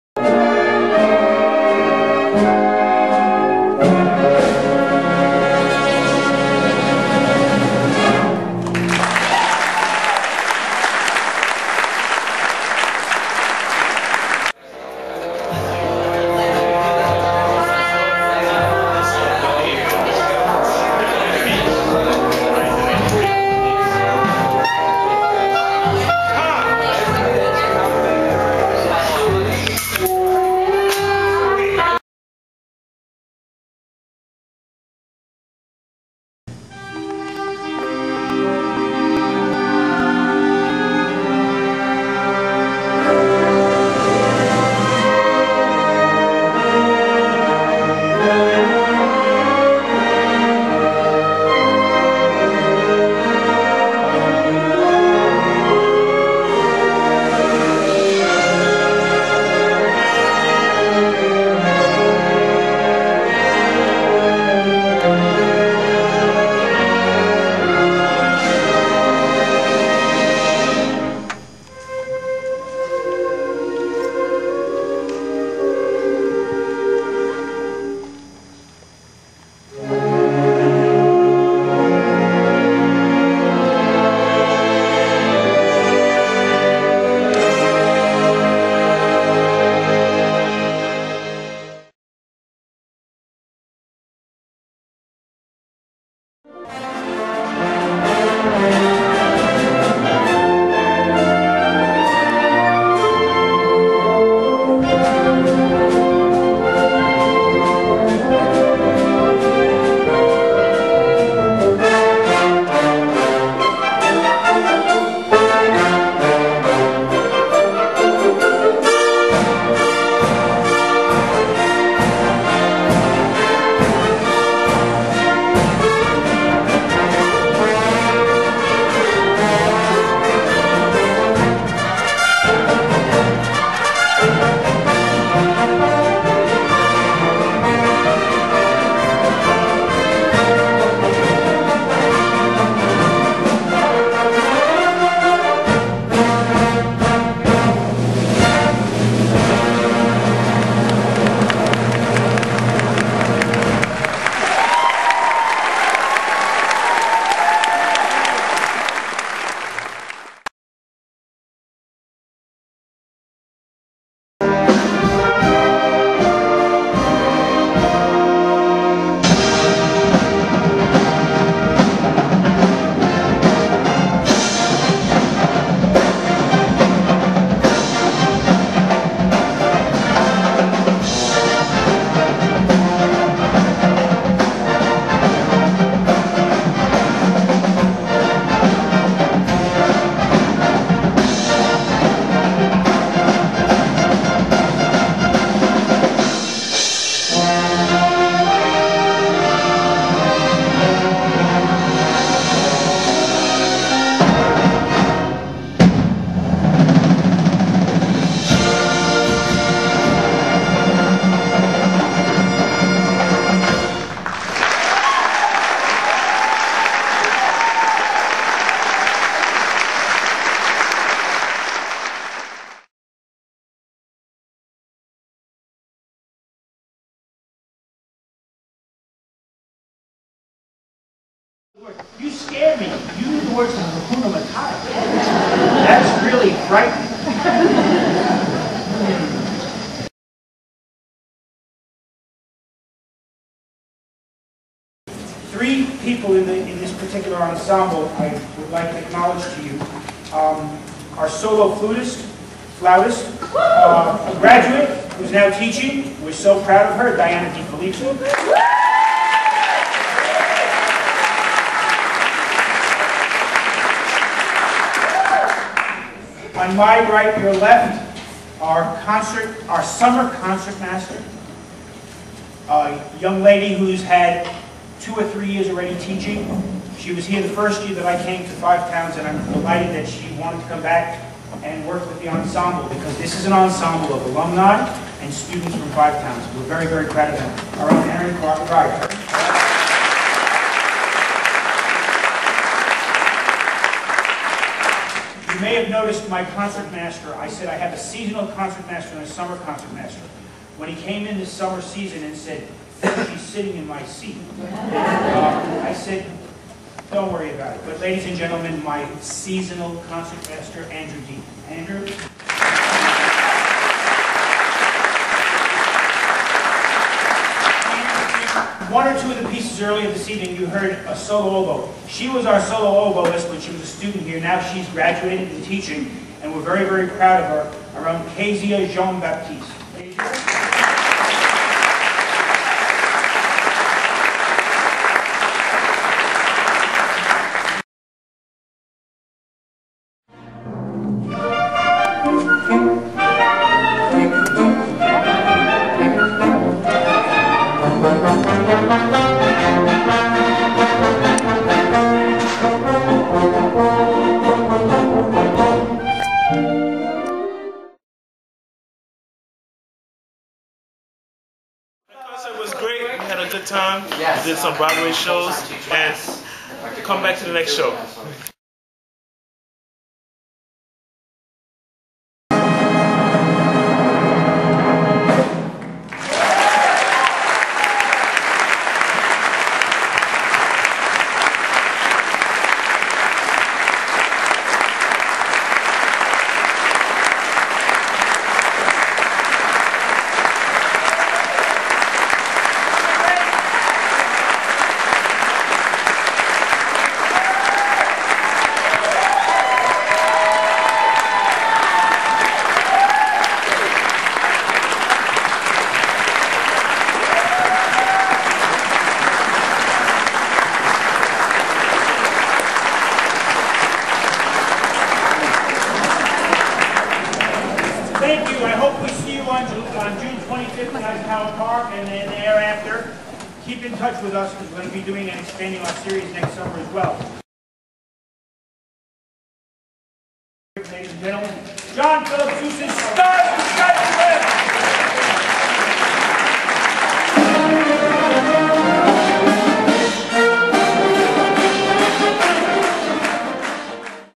FTC Pops: Summer Concert, June 11th
This video is from the concert, "Pops on Broadway", June 11, 2010 at DHPAC/Dix Hills Performing Arts Center at Five Towns College. Presenting..."Five Towns College Concert Pops". Conductor, Dean Karahalis. This is a highlight video, with musical excerpts. It also includes the on-stage introductions of principals: Diana DiFilippi, flute; Kezia Jean-Baptiste, oboe; Erin Krayer, clarinet; and Andrew Dietmann, clarinet. Fan Video by blogger Ki from DHPAC, who is serving as an embedded flutist/blogger for this summer season of pops concerts.